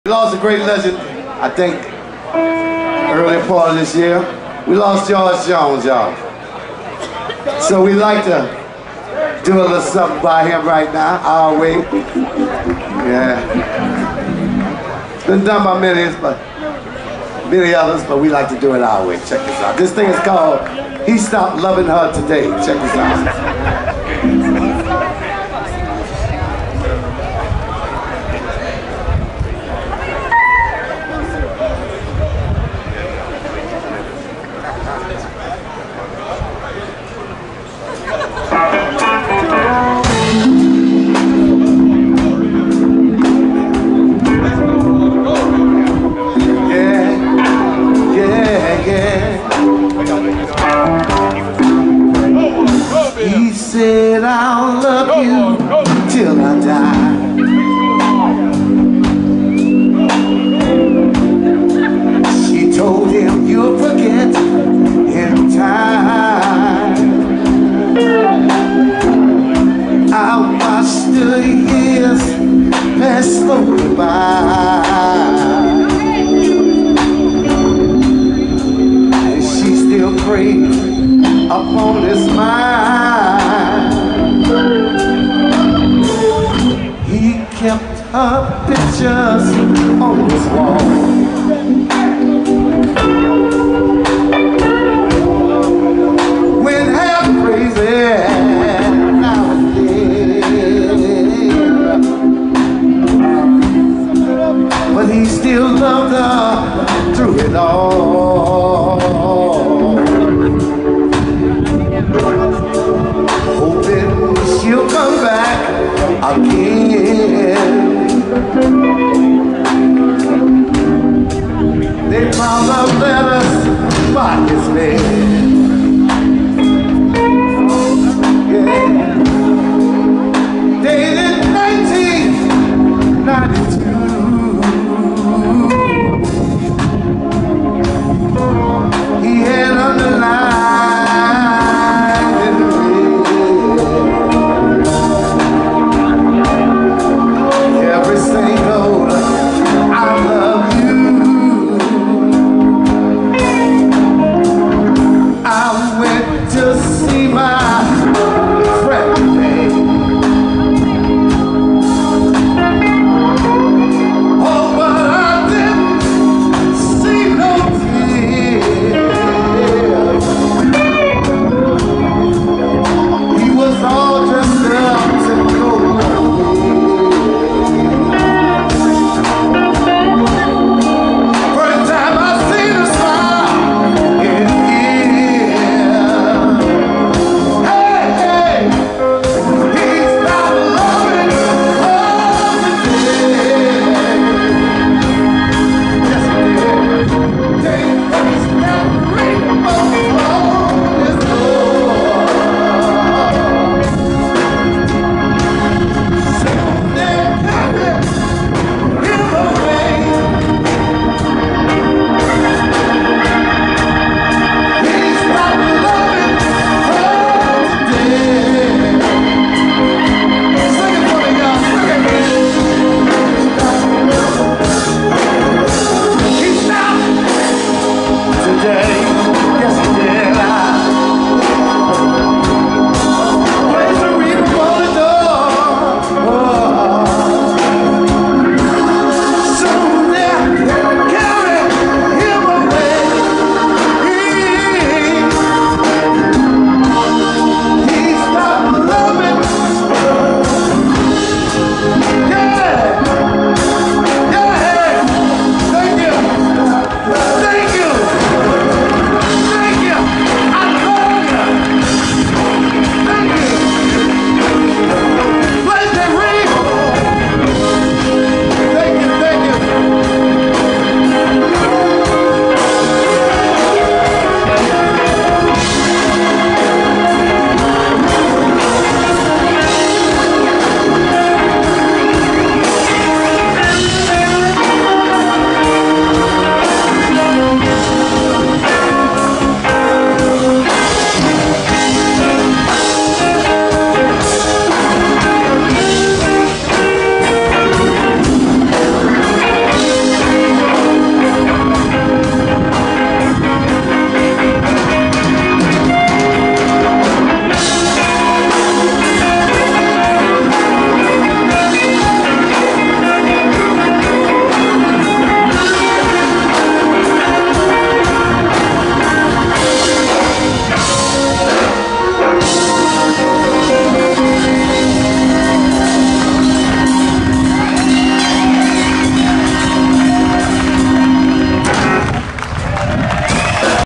Brother Tyrone and the Mind Benders- - NOLA Blues & BBQ Fest - October, 2014
The New Orleans Blues and BBQ Fest, Lafayette Square. This is amateur video. I do not monetize or profit in any way from it.  I post it to promote the artist and the live music venues. Tim